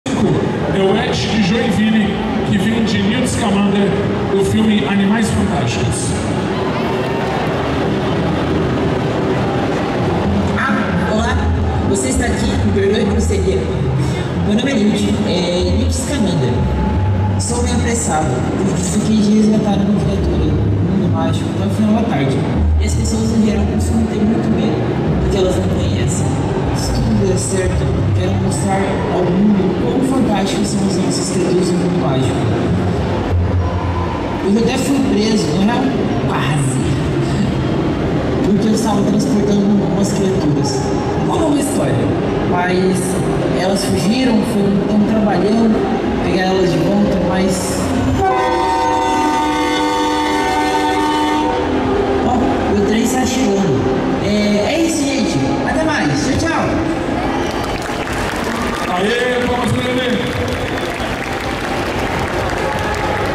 Anime Friends 2022 - Cosplay: Newt (tradicional)
Cosplay de Newt Scamander (Animais Fantásticos e Onde Habitam) no concurso de cosplay individual do Anime Friends 2022.